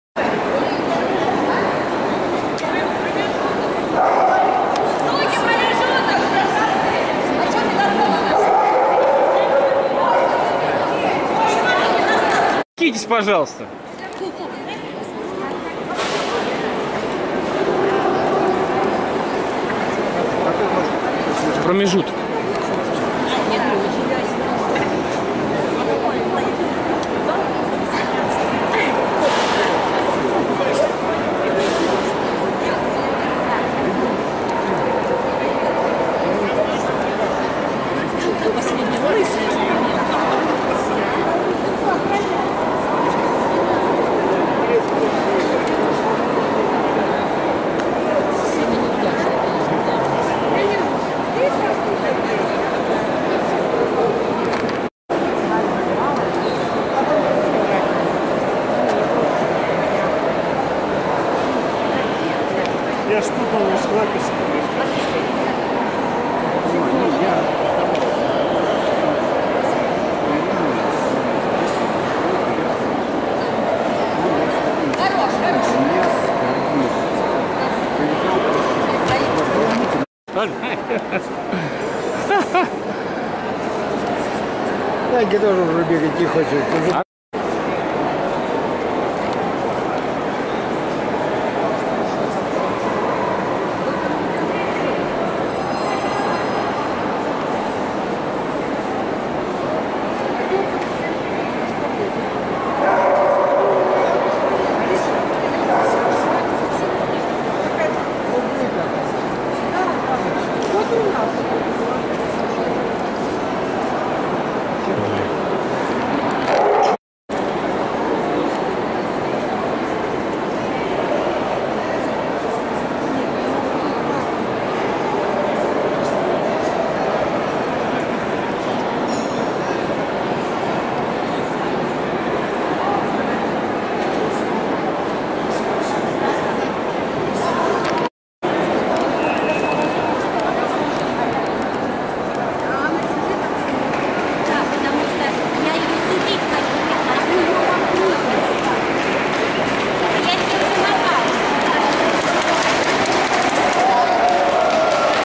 4 ИНТЕРНАЦИОНАЛЬНАЯ МОНОПОРОДНАЯ ВЫСТАВКА «КАВКАЗСКИЕ ОВЧАРКИ 2016»
СУКИ, КЛАСС
СУКИ, СУДЬЯ ЭКСПЕРТ: Г. КИРКИЦКАЯ.